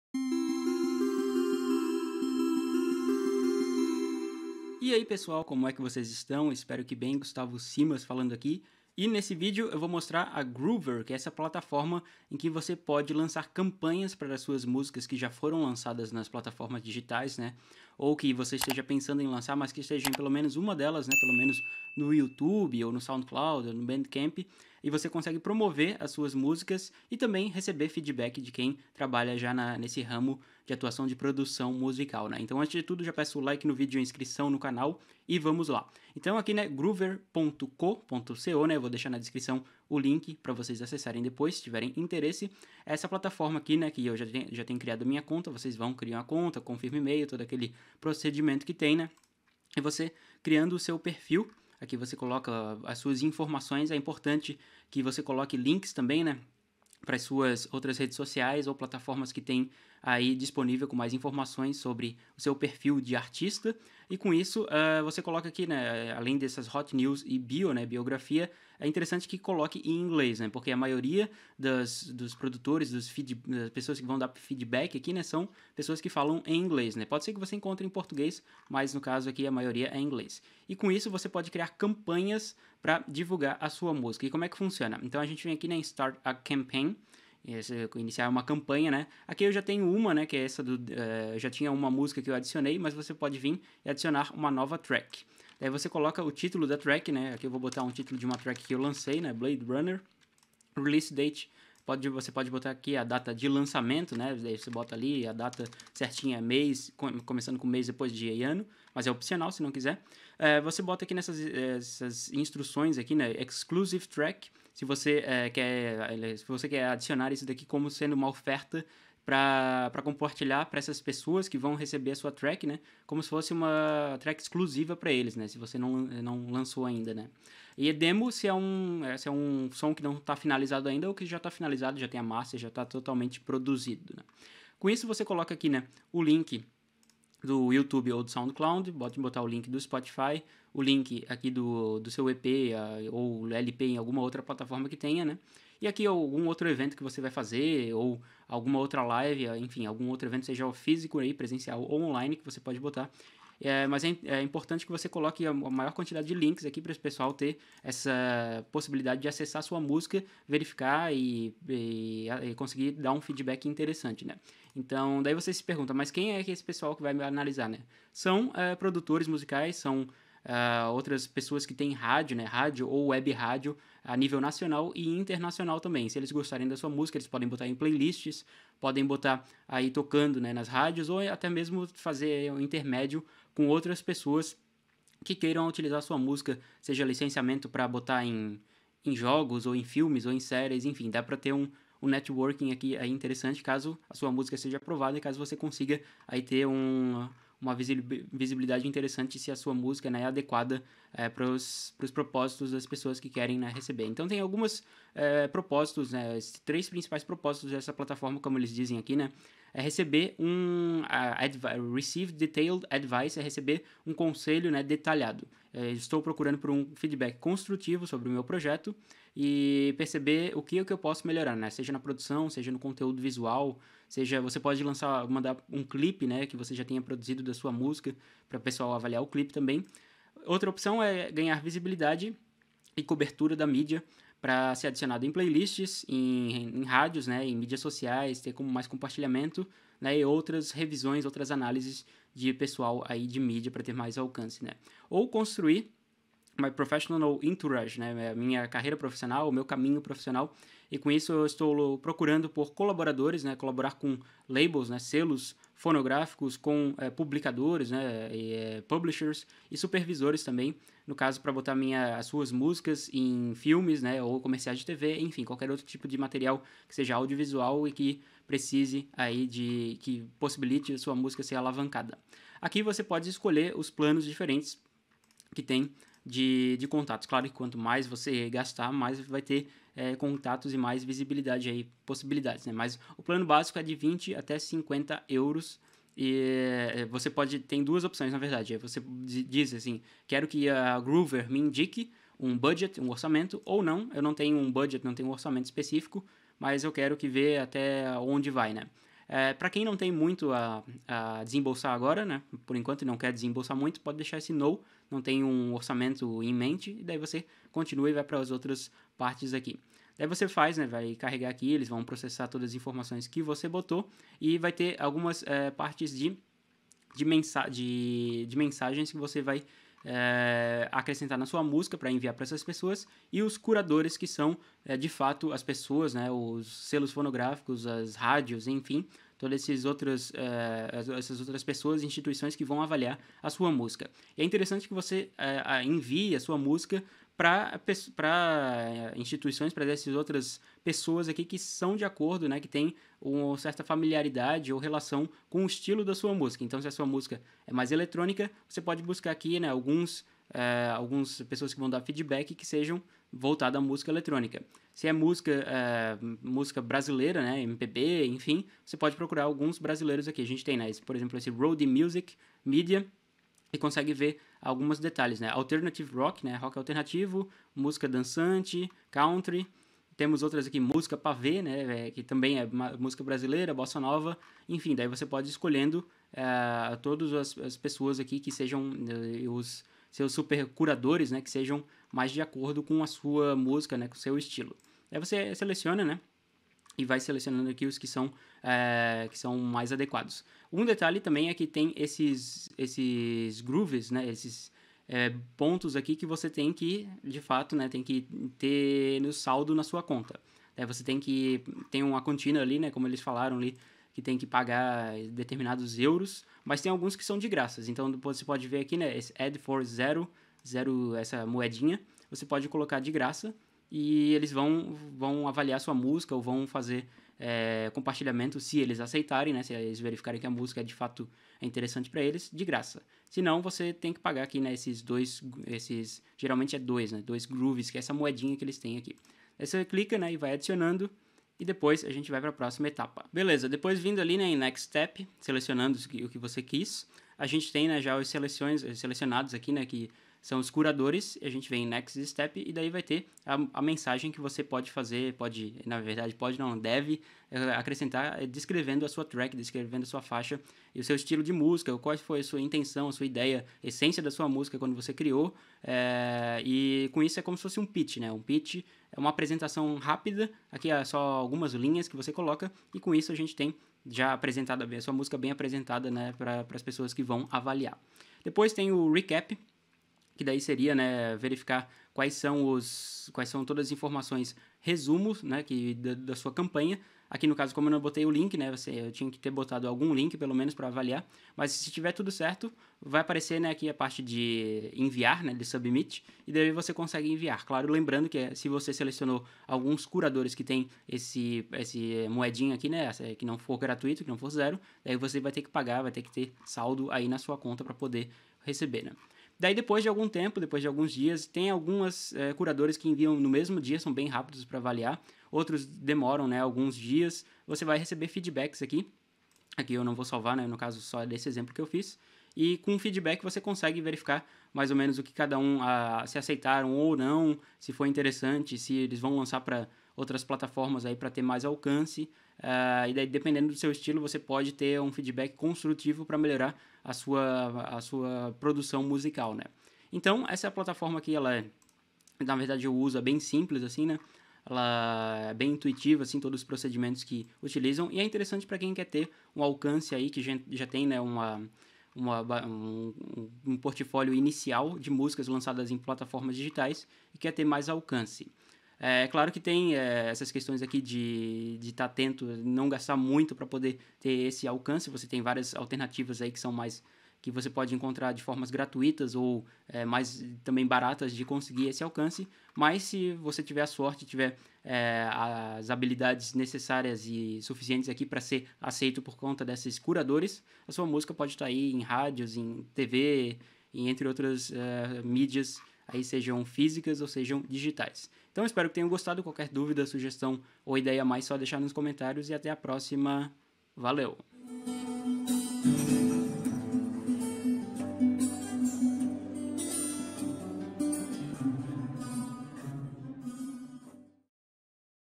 GROOVER | Alcance musical
Groover é um site onde você pode enviar sua música (já lançada em alguma plataforma digital, Youtube, Soundcloud, Spotify, etc.) para receber feedback e também ter a possibilidade de ampliar o alcance musical. Com curadores selecionados, sua música pode tocar em rádios, webrádios, realizar networking com profissionais do ramo musical, entre outras ações.

💵Apoio pelo Picpay: @simasgs